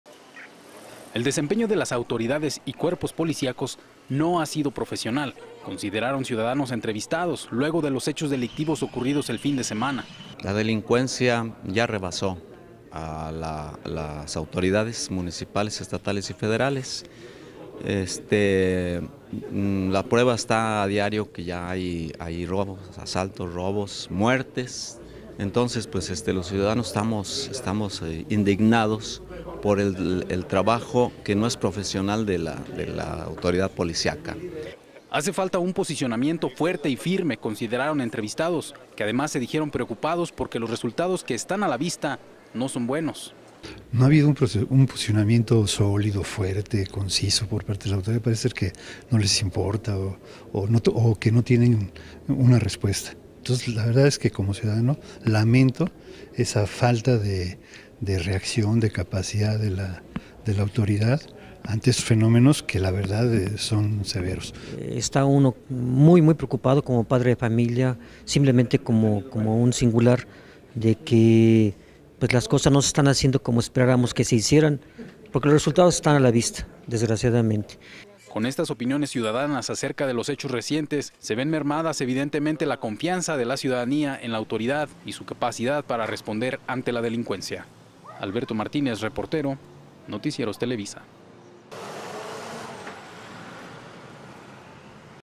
Bajio Noticias - Afecta crimen confianza en autoridades
El desempeño de las autoridades y cuerpos policiacos no ha sido profesional, consideraron ciudadanos entrevistados luego de hechos delictivos del fin de semana pasado.